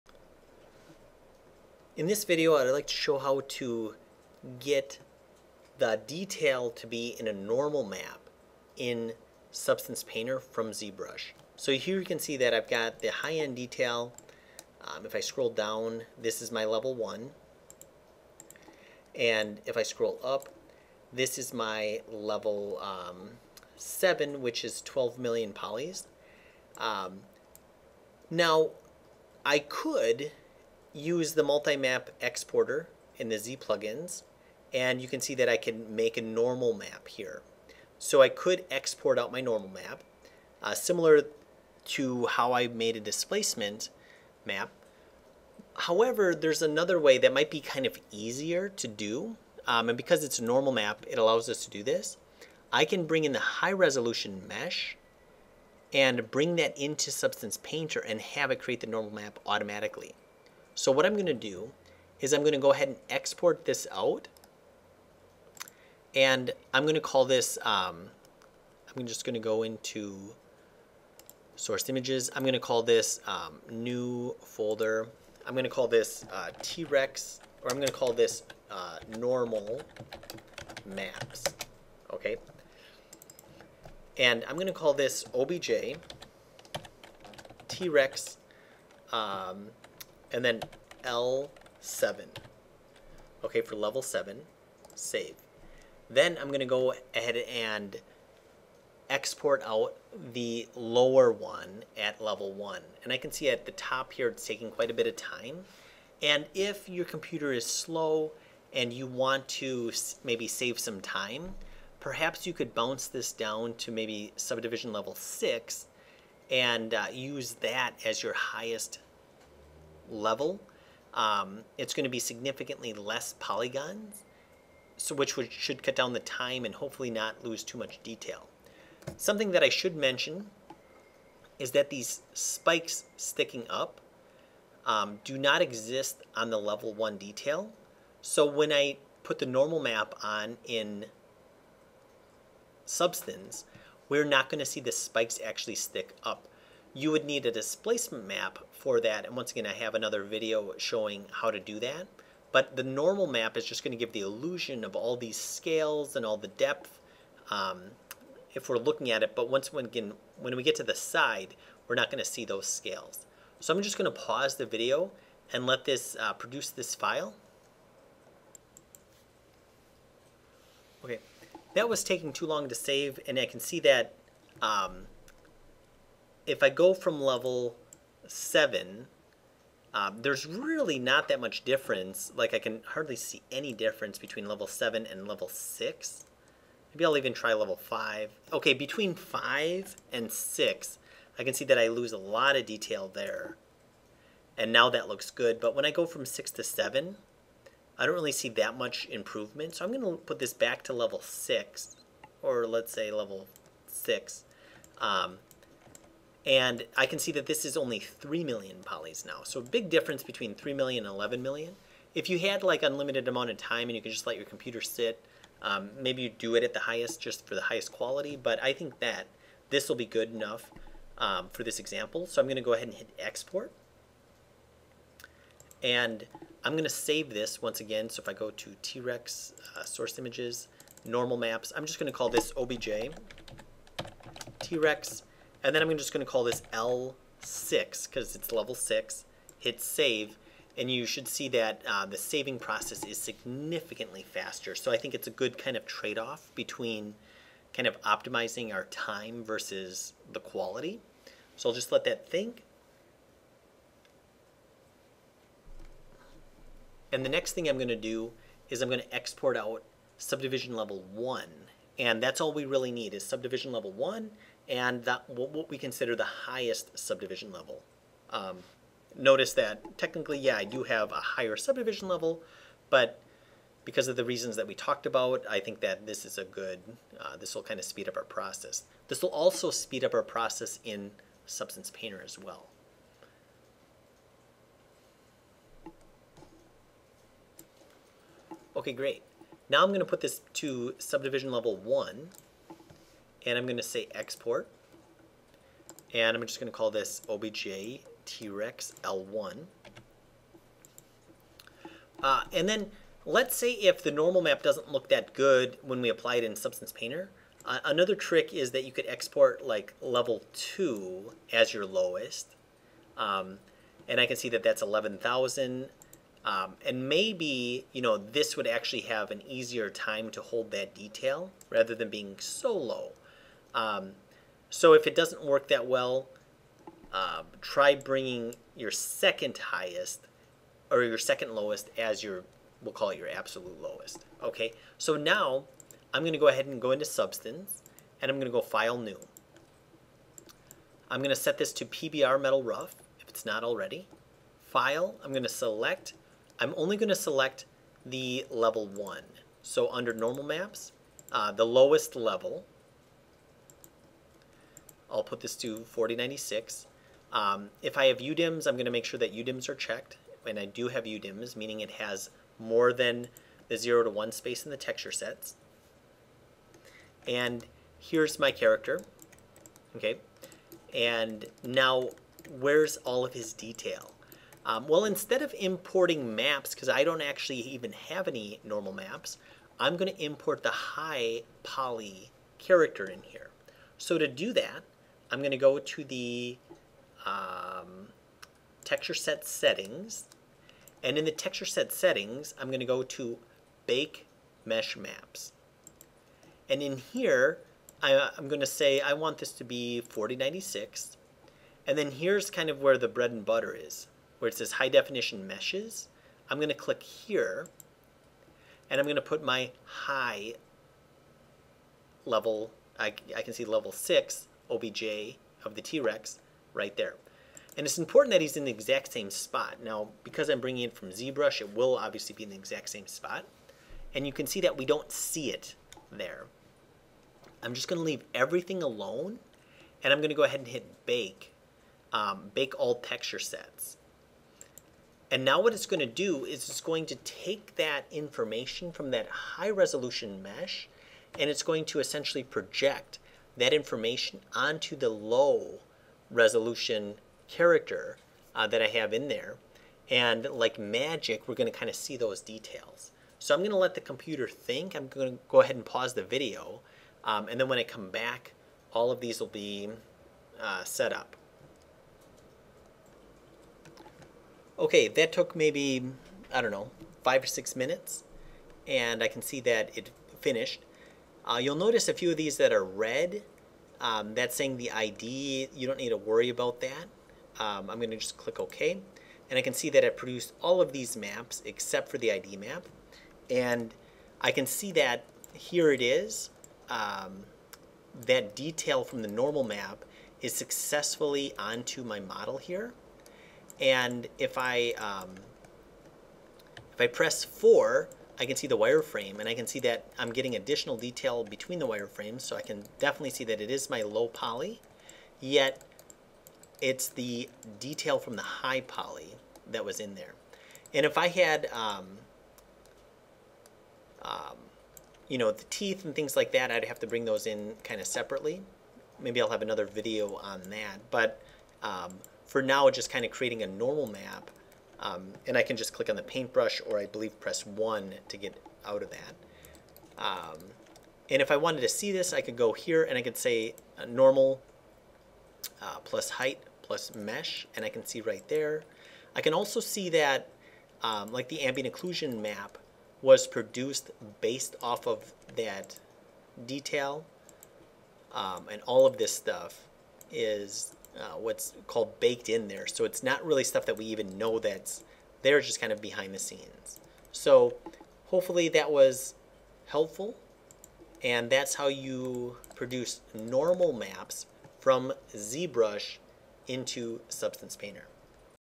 Creating Normal Maps in Substance Painter from ZBrush
Learn how to take your high resolution mesh in ZBrush and create a normal map in Substance Painter in this free tutorial by dcbittorf.  I will walk you though step-by-step how to bake your high resolution detail from ZBrush into Substance Painter using the Bake Mesh Maps.

TIMECODES:
0:00  Intro
1:05  Exporting High Res Mesh to Substance Painter
4:50  Exporting Low Res Mesh to Substance Painter
5:36  Importing Mesh Into Substance Painter
6:29  Baking Normals from High Res Mesh in Substance Painter